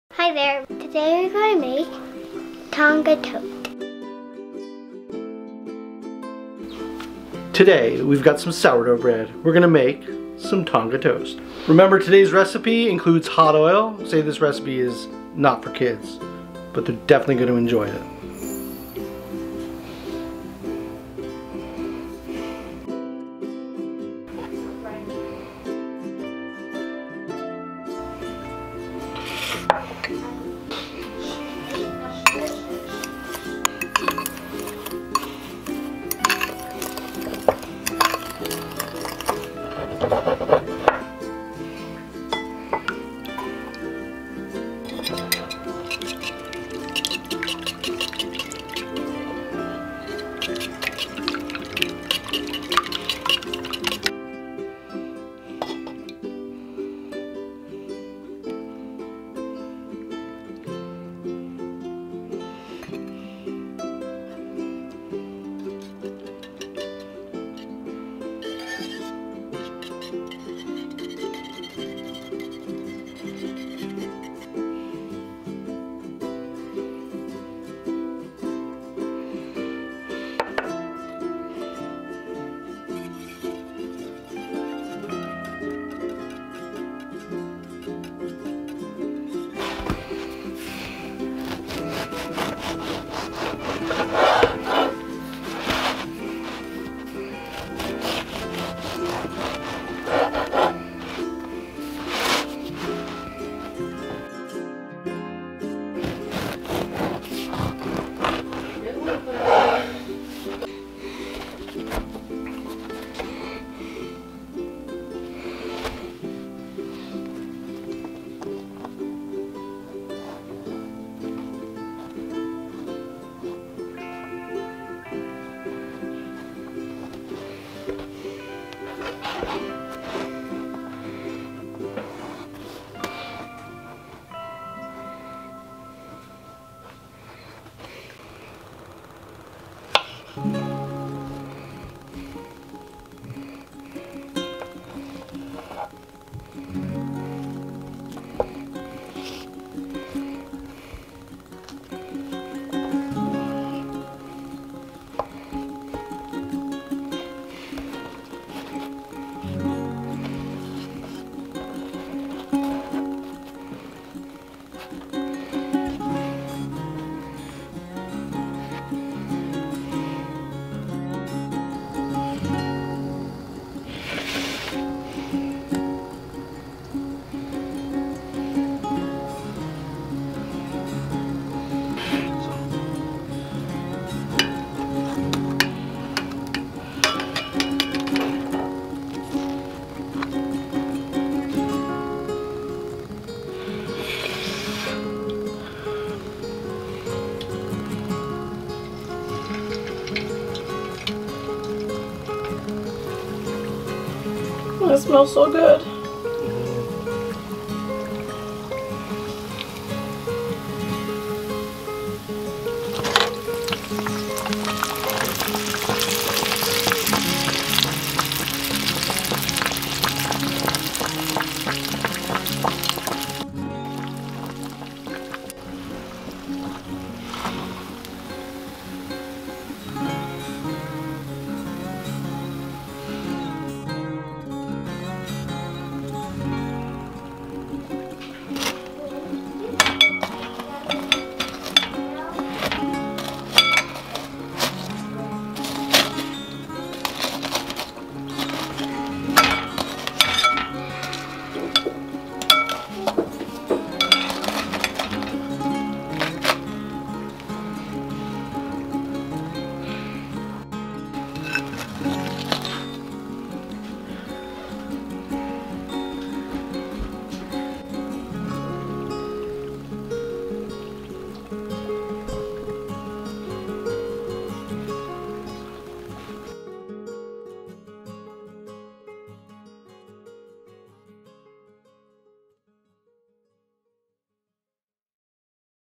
Tonga Toast Recipe a deep fried Disney at Home Adventure
We made Tonga toast at home and spoilers!! It tasted amazing.
Remember this recipe is not for kids. Enjoy.

✈✈✈✈✈✈Recommendations✈✈✈✈✈✈

Camera - Canon M50:

Tripod Fotopro UFO2:

Rode VideoMic GO Light Weight On-Camera Microphone:

DJI Ronin-SC - Camera Stabilizer, 3-Axis Handheld Gimbal